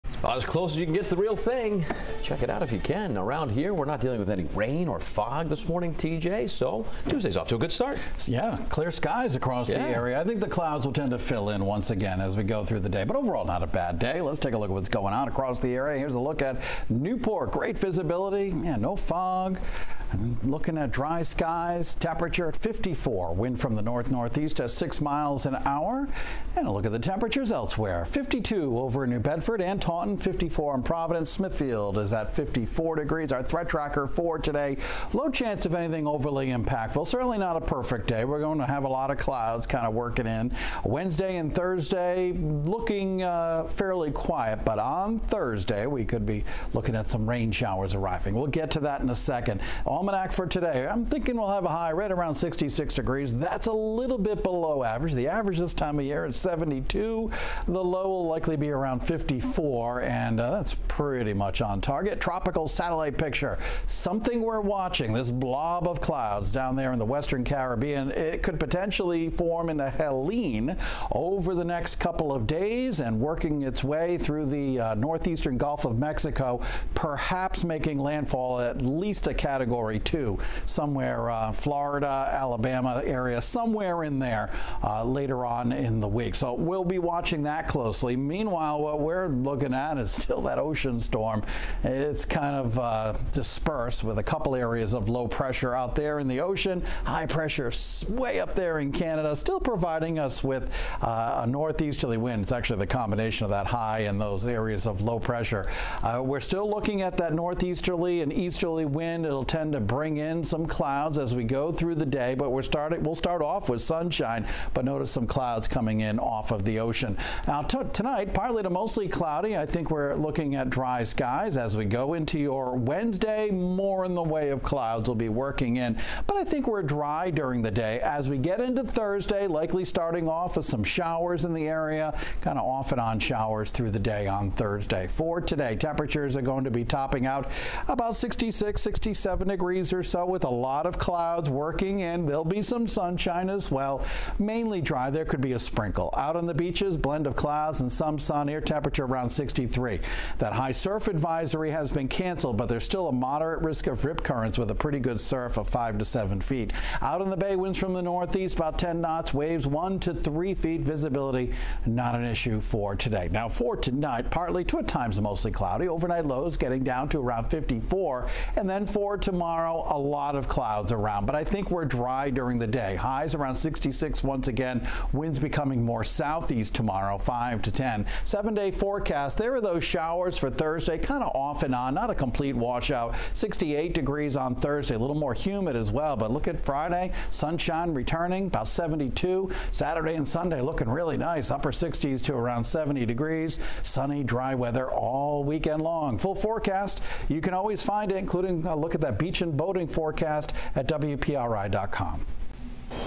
WPRI 12 Weather Forecast for 9/24/24: Lots of clouds today with some sunshine; showers ahead
WPRI 12 Weather Forecast for 9/24/24 with T.J. Del Santo:  Lots of clouds today with some sunshine; showers ahead